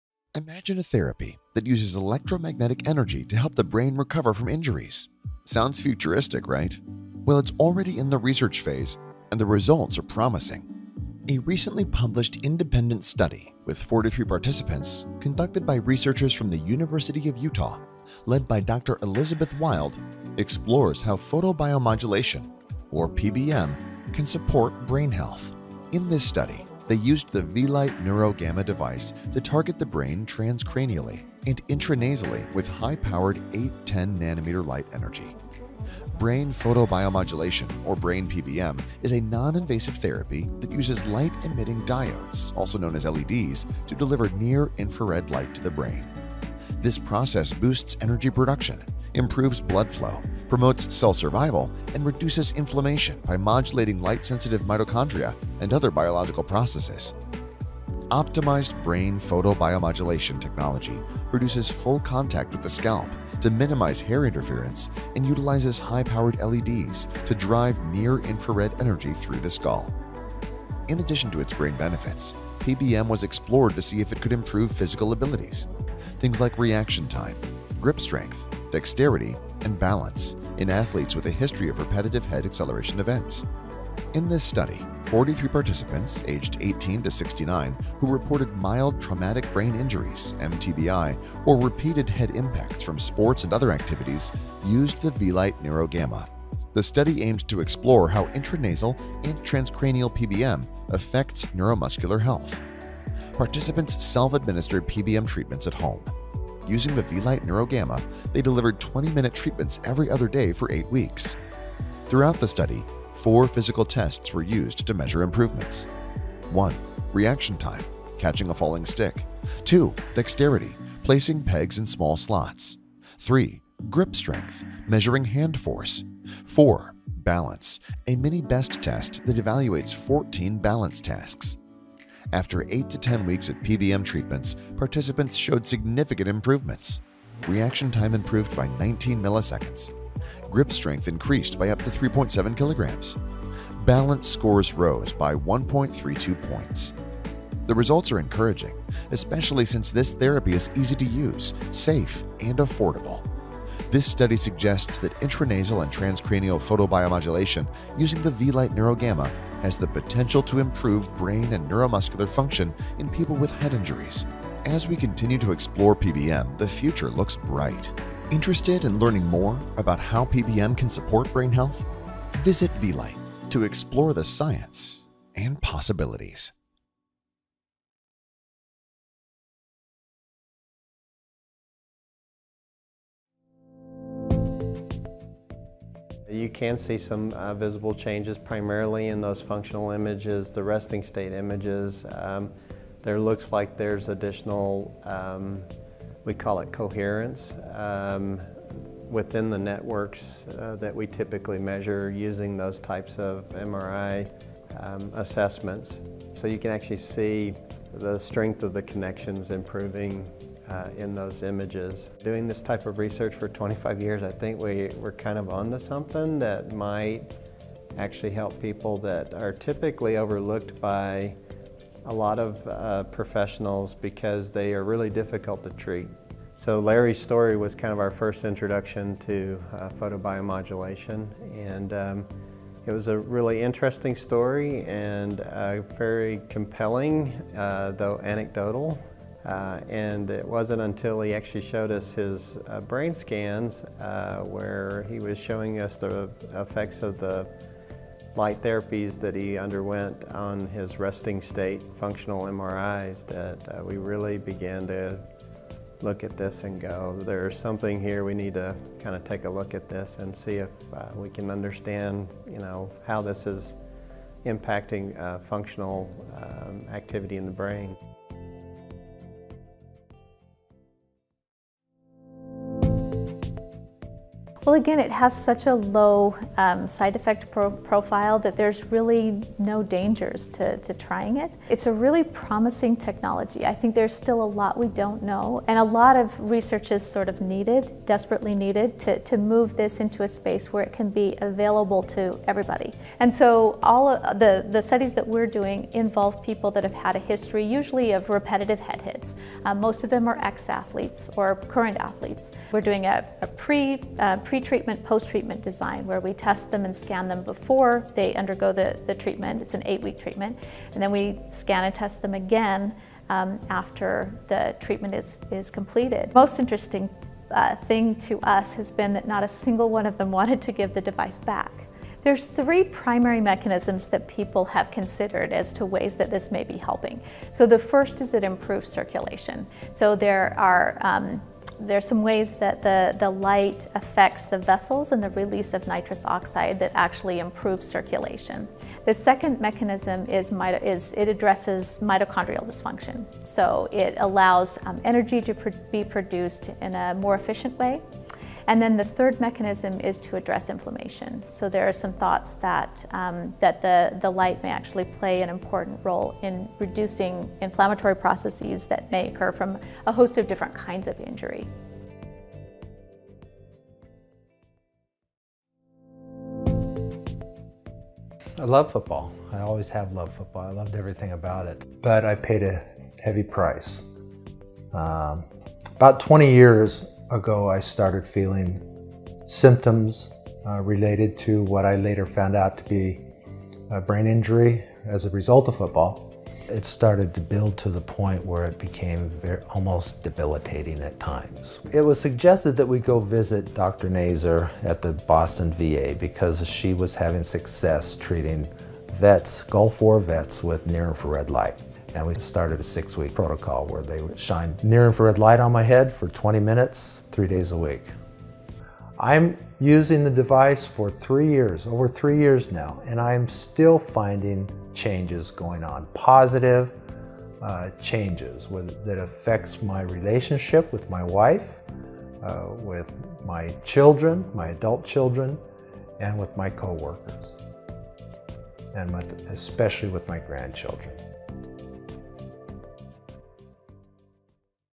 TBI Photobiomodulation Clinical Trial Results | Researcher Interviews | U of Utah | Vielight Neuro
Brain photobiomodulation (PBM) with the Vielight Neuro Gamma was explored as a therapy for brain injuries by neurology professors from the University of Utah because of the promise shown by Vielight's patented simultaneous intranasal/transcranial PBM technology and Vie-LED technology.

Brain PBM utilizes light-emitting diodes (LEDs) to target brain regions with near-infrared light, promoting energy production, blood flow, and cell survival while reducing inflammation. This therapy may also enhance neuromuscular health by improving reaction time, dexterity, grip strength, and balance.

This study enrolled 49 participants (43 completed) aged 18-69 years, all with self-reported mTBI or RHAEs from sports or other activities. The study aimed to examine the effects of itPBM on neuromuscular health and found potential benefits, suggesting that PBM might improve or preserve physical performance and neuromuscular function.